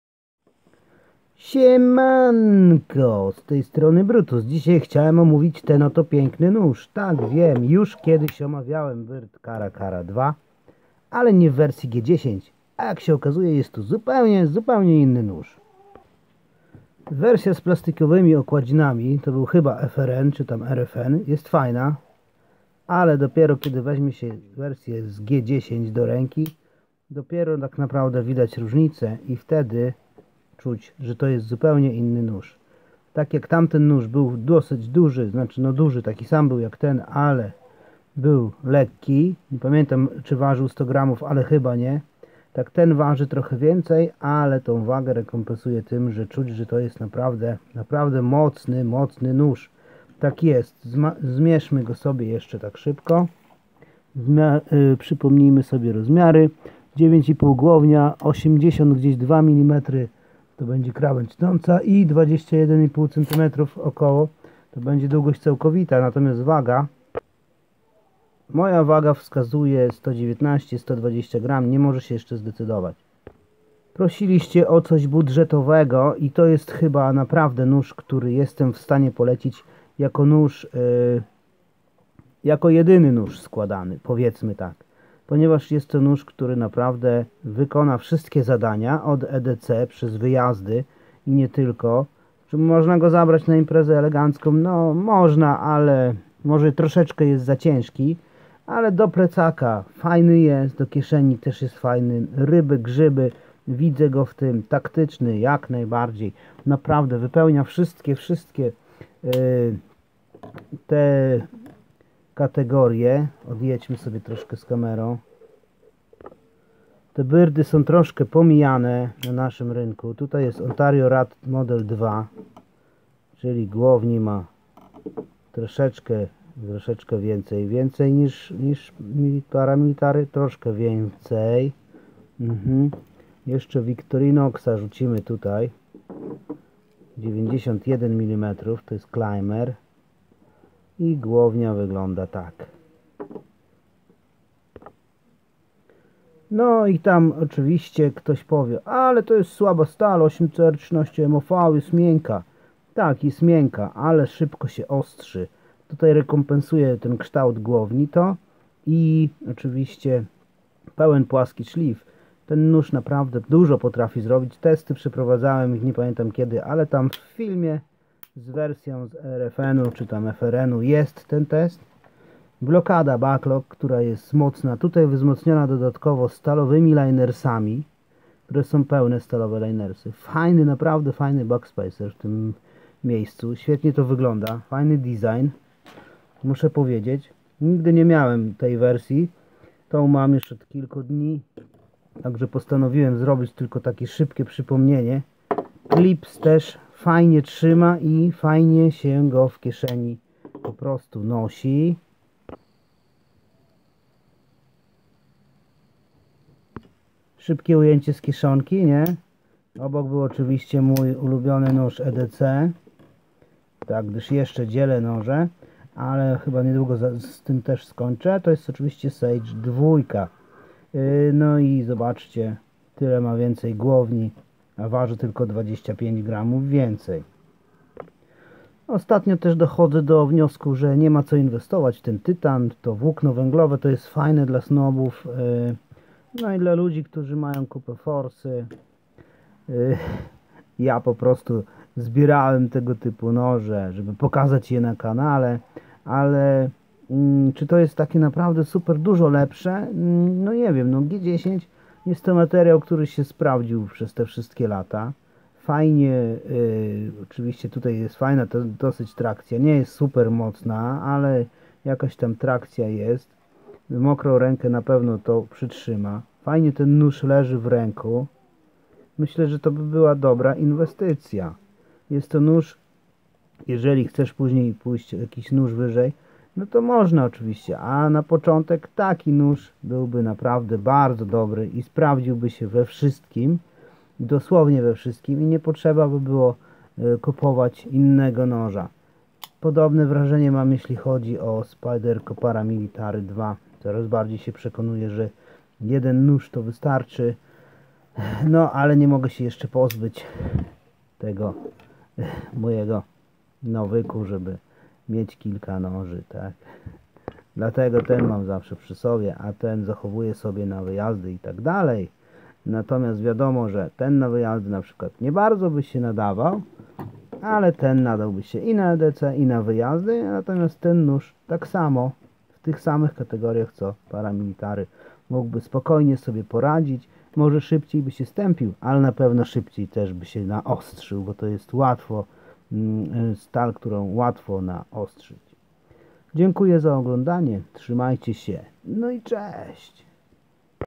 Byrd Cara Cara 2 G10 recenzja
Mówiąc jedyny składany nóż godny polecenia mam na myśli nóż, który sprawdzi się we wszystkich zadaniach. Co nie znaczy, ze nie ma innych noży które w tej roli się sprawdzą.